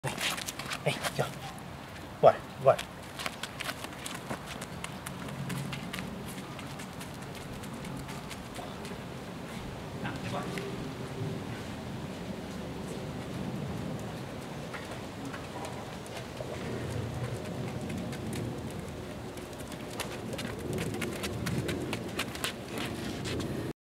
GABRIELLA - Br Lake Park Gabriella - III
GABRIELLA - Br Lake Park Gabriella - Basset Hound female 9 months old - "My first day of training" (Multi BIS BISS Ch. Lake Park Thor X Park Melody Last Chance)